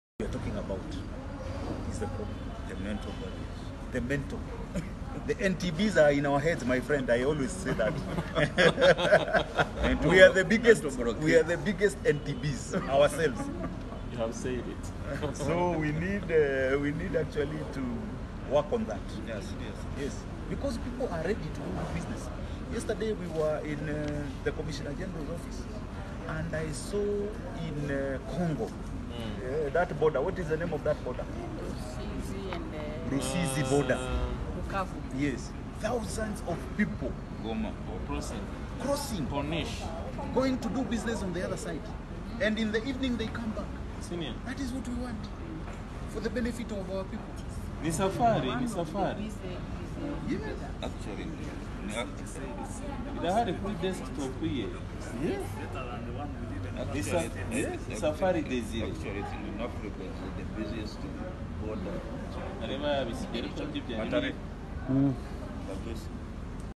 EALA Chairman Hon.Simon Mbugua Talks of Removal of Non-Tariff Barriers
It is with no doubt that if all Non-Tariff Barriers (NTBs) hindering trade within the EAC Common Market are removed, the domestic demand and market of over 150 million people from the six EAC partner states will attract investments from all over the world as it will be more economically and viable for all Investors to invest in the EAC.